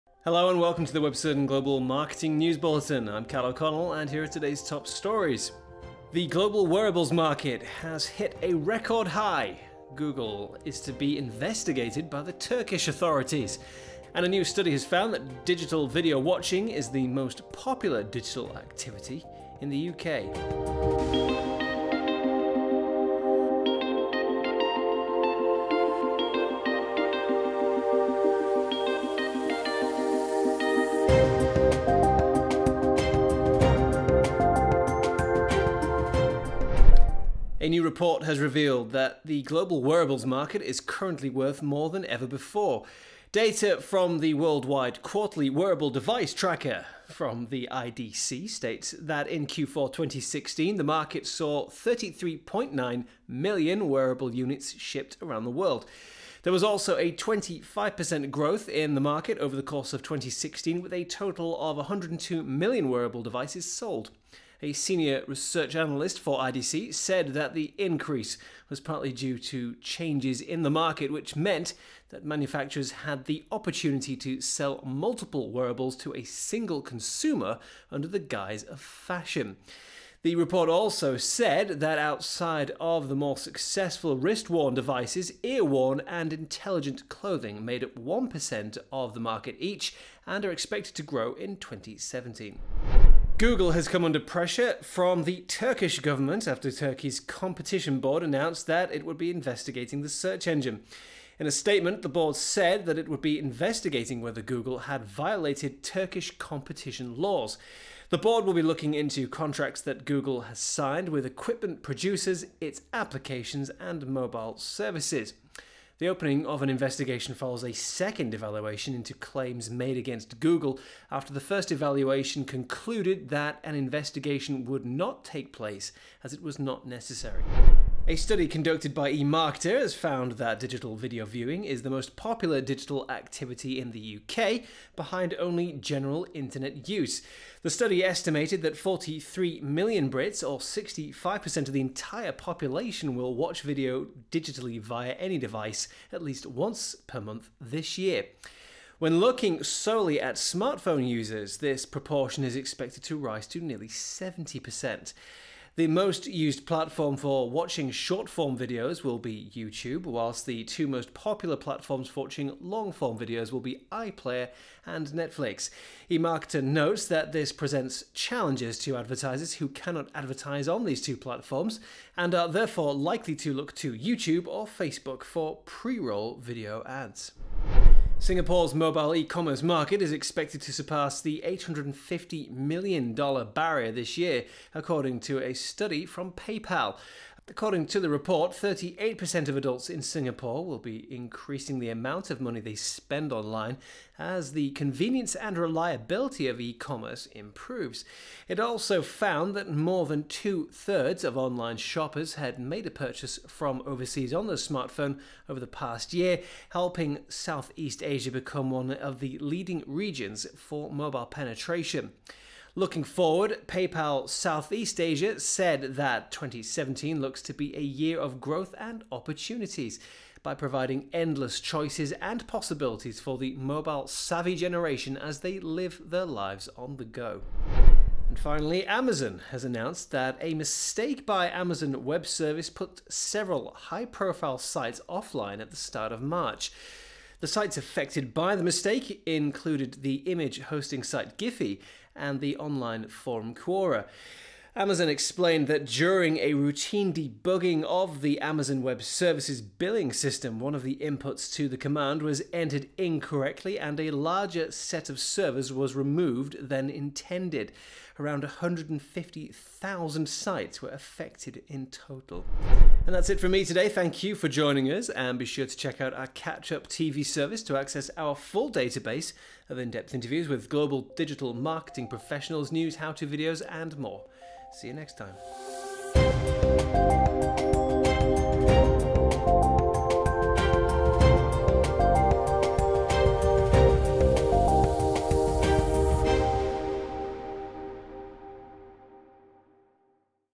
Global Wearables Market Hits New Heights
A new report has revealed that the global wearables market is currently worth more than ever before.

Data from the Worldwide Quarterly Wearable Device Tracker from the IDC states that in Q4 2016 the market saw 33.9 million wearable units shipped around the world.

There was also a 25% growth in the market over the course of 2016, with a total of 102 million wearable devices sold.

A senior research analyst for IDC said that the increase was partly due to changes in the market which meant that manufacturers had the “opportunity to sell multiple wearables to a single consumer under the guise of fashion”.

The report also said that outside of the more successful wrist-worn devices, ear-worn and intelligent clothing made up 1% of the market each, and are expected to grow in 2017.

Google has come under pressure from the Turkish government, after Turkey’s Competition Board announced that it would be investigating the search engine.

In a statement, the board said that it would be investigating whether Google had violated Turkish competition laws.

The board will be looking into contracts that Google has signed with equipment producers, its applications, and mobile services.

The opening of an investigation follows a second evaluation into the claims made against Google, after the first evaluation concluded that an investigation would not take place as it was not necessary.

A study conducted by eMarketer has found that digital video viewing is the most popular digital activity in the UK, behind only general internet use.

The study estimated that 43 million Brits, or 65% of the entire population, will watch video digitally via any device at least once per month this year.

When looking solely at smartphone-users, this proportion is expected to rise to nearly 70%.

The most used platform for watching short-form videos will be YouTube, whilst the two most popular platforms for watching long-form videos will be iPlayer and Netflix.

eMarketer notes that this presents challenges to advertisers, who cannot advertise on these two platforms, and are therefore likely to look to YouTube or Facebook for pre-roll video ads.

Singapore’s mobile ecommerce market is expected to surpass the 850-million-dollar barrier this year, according to a study from PayPal.

According to the report, 38% of adults in Singapore will be increasing the amount of money they spend online, as the convenience and reliability of ecommerce improves.

It also found that more than two thirds of online shoppers had made a purchase from overseas on their smartphone over the past year, helping South East Asia to become one of the leading regions for mobile penetration.

Looking forward, PayPal South East Asia said that “2017 looks to be a year of growth and opportunities” by providing “endless choices and possibilities for the mobile-savvy generation as they live their lives on the go”.

And finally, Amazon has announced that a mistake by Amazon Web Service put several “high-profile” sites offline at the start of March.

The sites affected by the mistake included the image hosting site Giphy and the online forum Quora.

Amazon explained that during a routine de-bugging of the Amazon Web Services billing system, “one of the inputs to the command was entered incorrectly and a larger set of servers was removed than intended”.

Around 150,000 sites were affected in total.